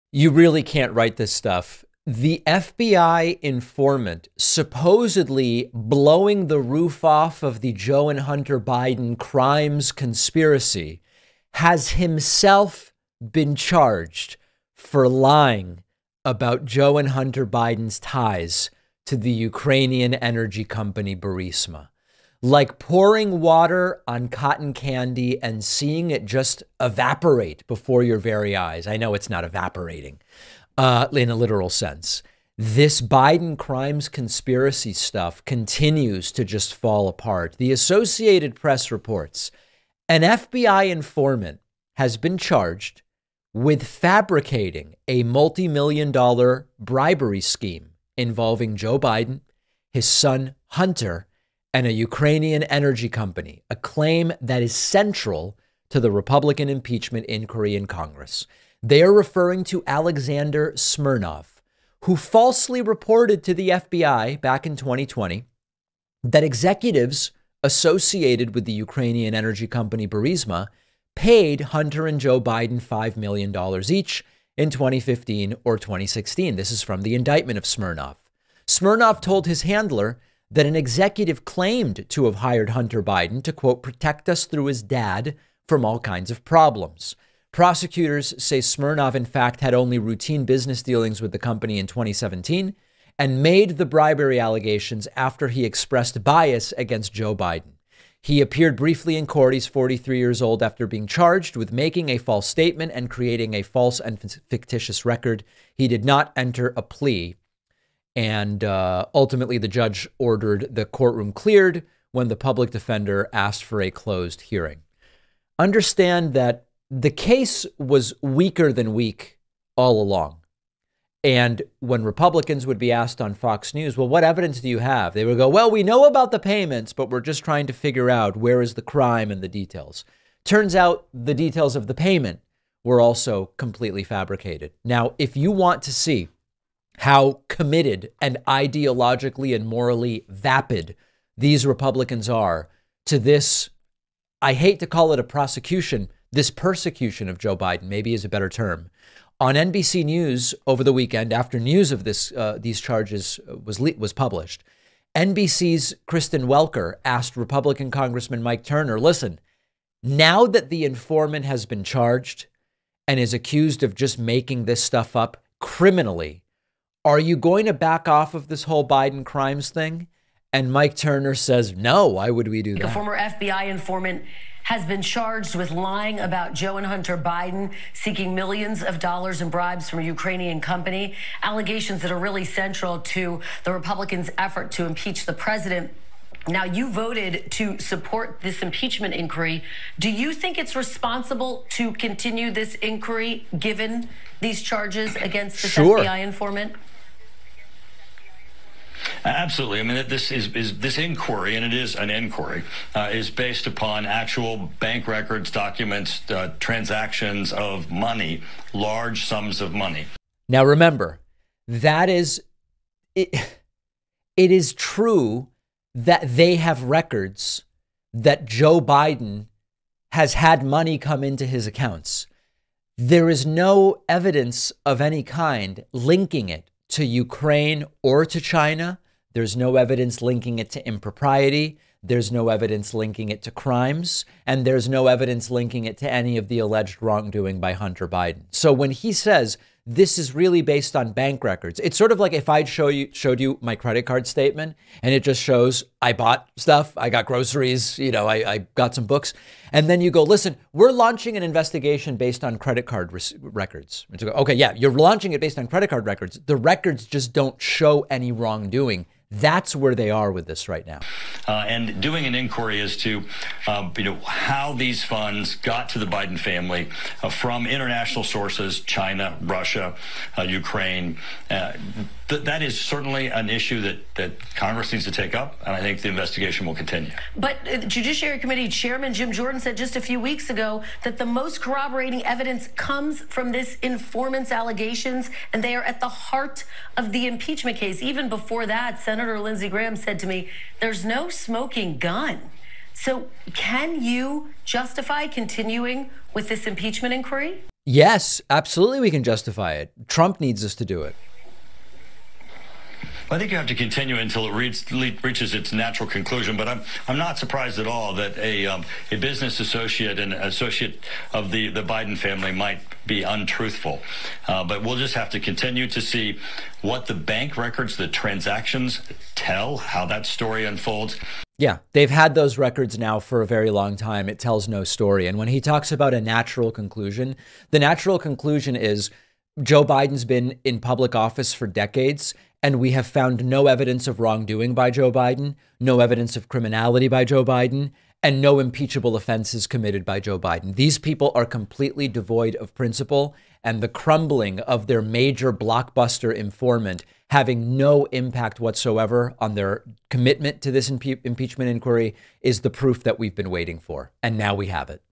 LOL: Biden crimes "informant" charged with crimes!
-- The Biden crimes fiasco falls apart as a supposed informant, Alexander Smirnov, has been himself charged with crimes

Leave a Voicemail Line: (219)-2DAVIDP

Broadcast on February 19, 2024